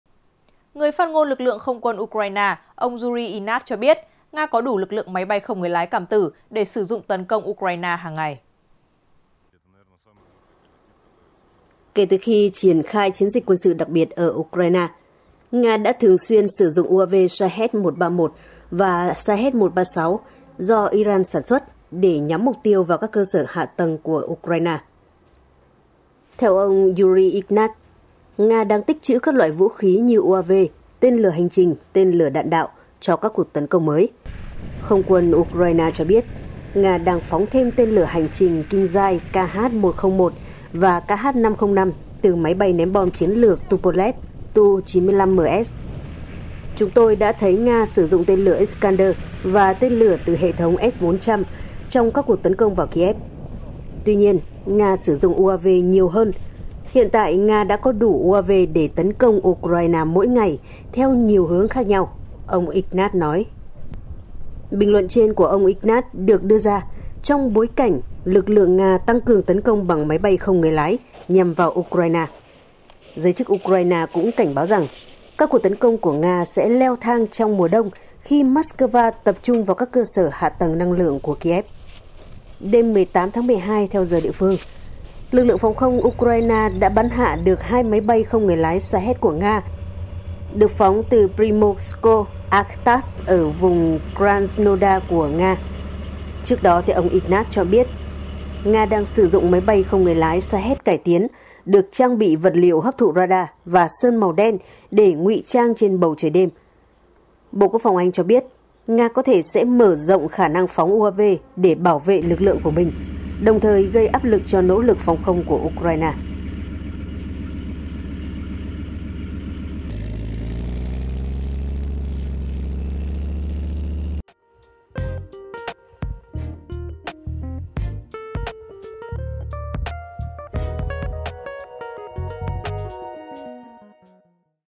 Ukraine nói Nga có đủ UAV cảm tử để tấn công hàng ngày | Báo Điện tử VOV
Báo Điện tử VOV | Người phát ngôn Lực lượng Không quân Ukraine, ông Yuri Ignat, cho biết, Nga có đủ lực lượng máy bay không người lái (UAV) cảm tử để sử dụng tấn công Ukraine hàng ngày.

37 Bà Triệu, Hoàn Kiếm, Hà Nội, Việt Nam